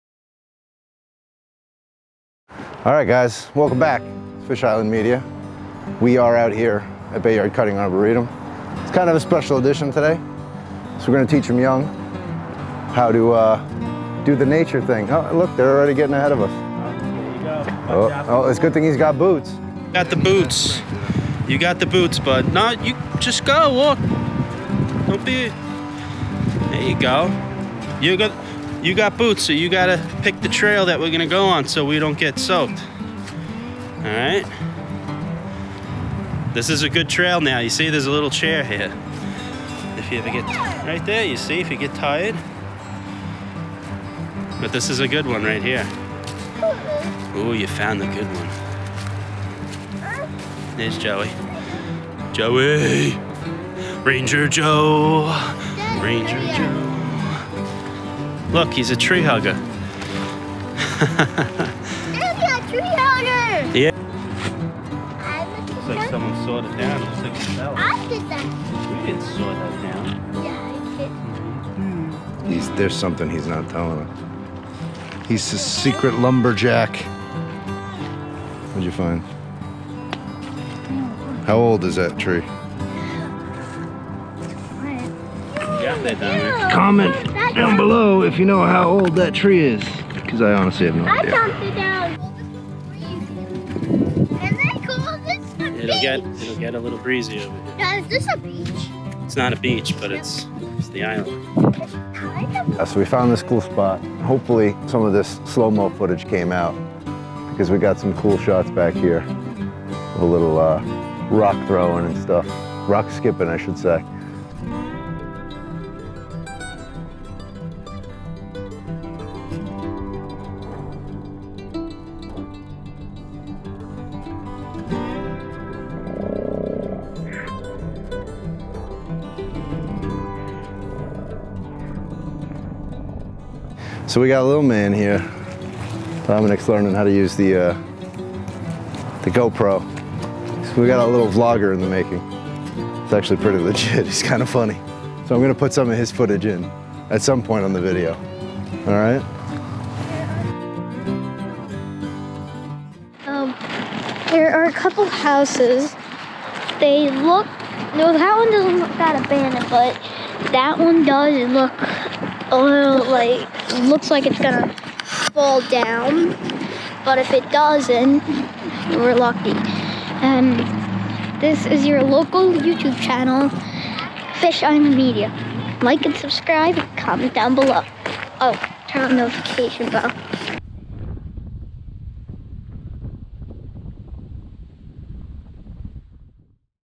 Checking out Bayard Cutting Arboretum in the Winter
Welcome back to fish island media.
Brad and I took his sons, Dominic and Ryan on a hike through Bayard Cutting Arboretum.
This is a pretty cool place during the summer, but its a whole lot different in the winter.
We tried to teach them to skip rocks, and managed to get some cool slowmo footage.
Thought we would show them early on, to enjoy the outdoors.
Turns out Dominic is a junior vlogger in training.
He just may become the next fresh face of fish island media.